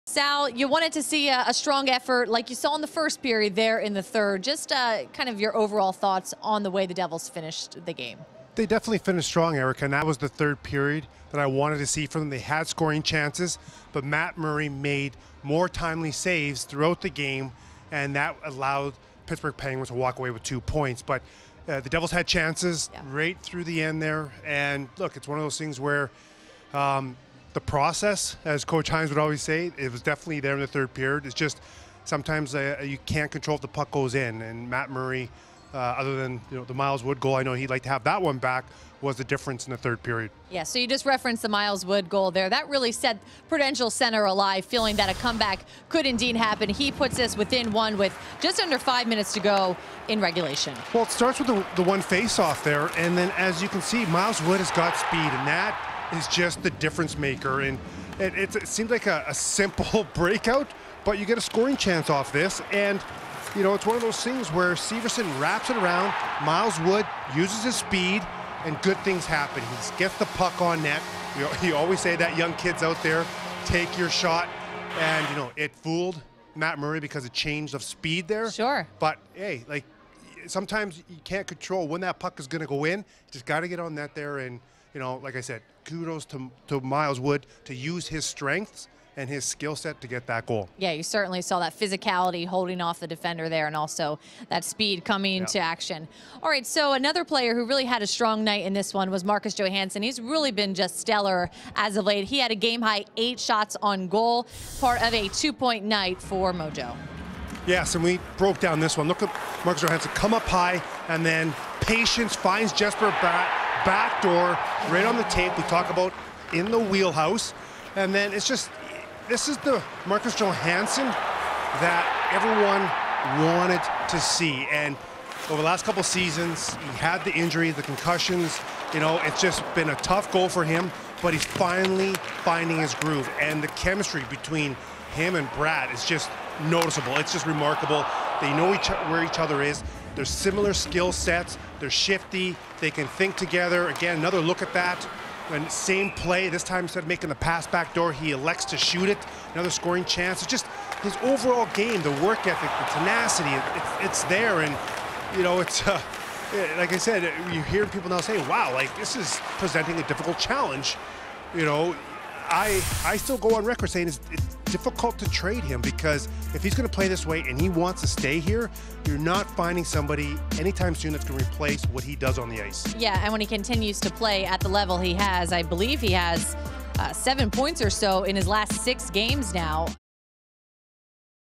Miles Wood and Marcus Johansson Put In Strong Shifts vs. Penguins | New Jersey Devils Postgame
Erika Wachter and Bryce Salvador take a closer look at the performances from Miles Wood and Marcus Johansson against the Penguins.